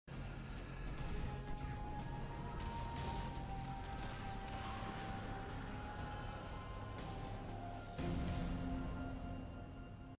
Malfeasance FTW
I am SO glad I got those 5 shots off before I got sucked up! I was sure I was going to have to go through another cycle!